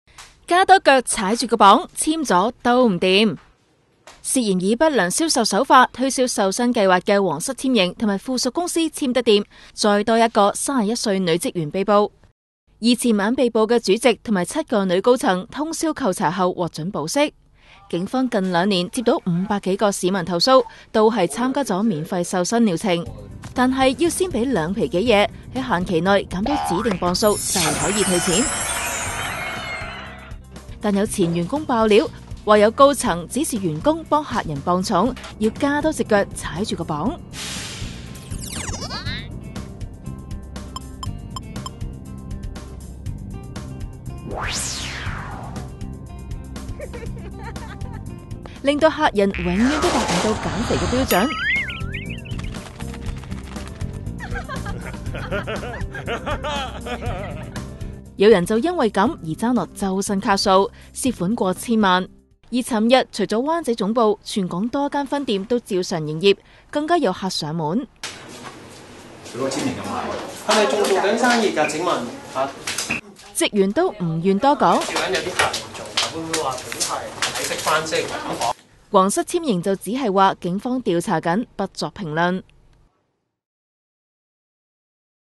加多腳踩住個磅 纖極唔掂
【本報訊】惹上詐騙案官非的皇室纖形集團風波未了，警方昨晚再拘捕一名女職員助查，初步發現中招市民逾 500 人，涉款逾 1,000 萬元。被捕纖形集團主席經扣查逾 30 小時後，昨晚與同案七名高層獲准保釋。有該公司前員工踢爆，曾接高層指示，替纖體療程參加者量度體重時，要「加多隻腳踩住個磅」，令參加者永遠無法達標，繼續付錢參加療程。
記者：卓永強　劉柏麟